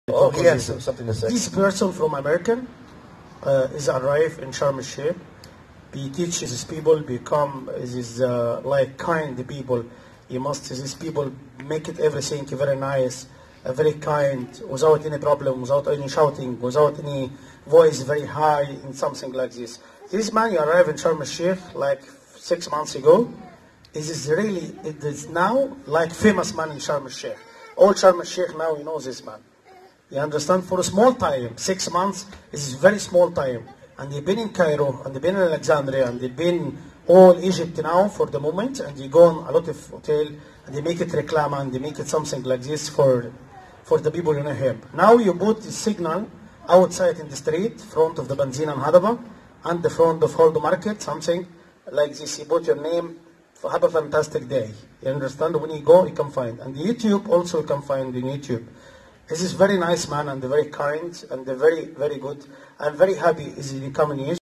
Please invite Dr. Fantastic to give his kindness speech to your school or company
Dr. Fantastic loves sharing the three commitments to kindness.